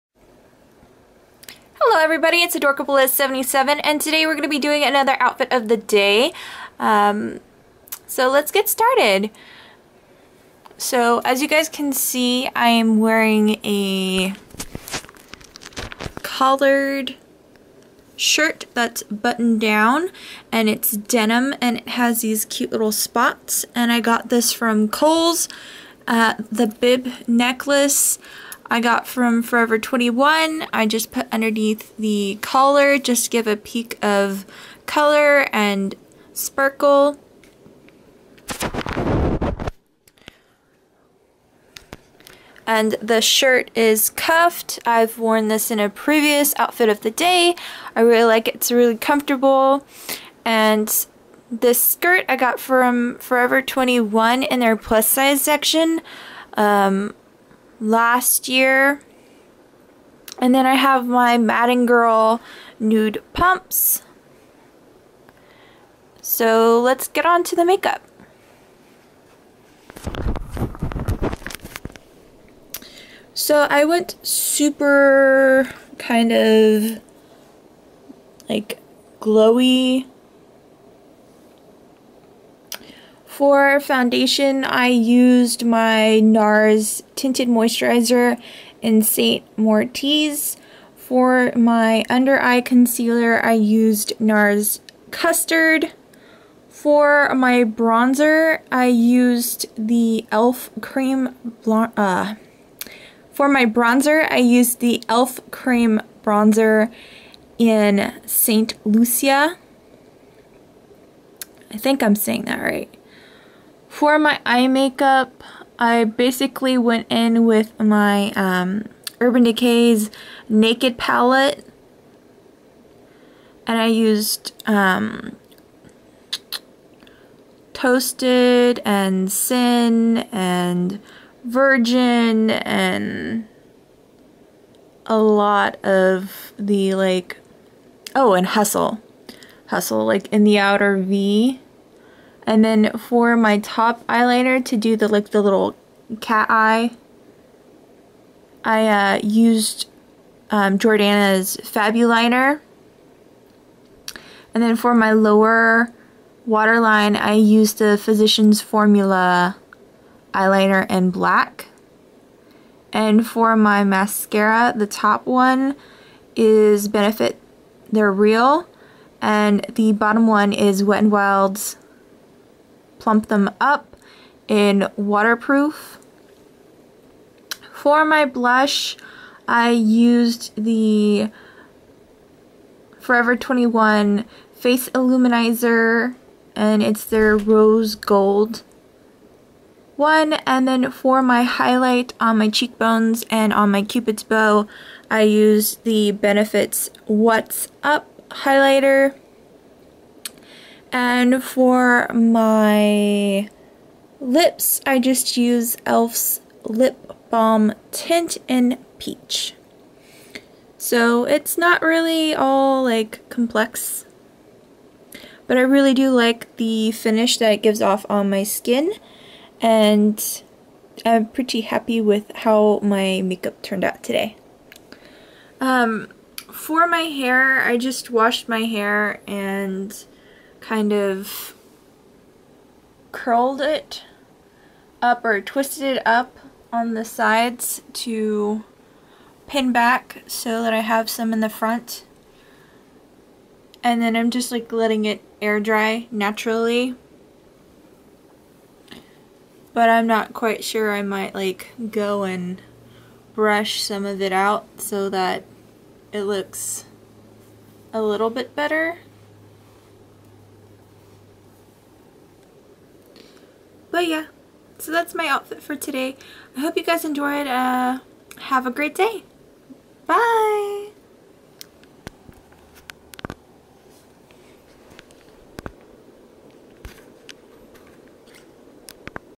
OOTD Denim & Lace
I felt pretty even if my hair just looked blah (I ended up blow drying it and brushing it out more). lol I have a problem with the color coral I'm a tad bit obsessed right now. I went girlie but my boyfriend got the country girl vibe off this outfit most likely due to the shirt. I'm in the process of loosing weight yay! So things are starting to fit loose or just better.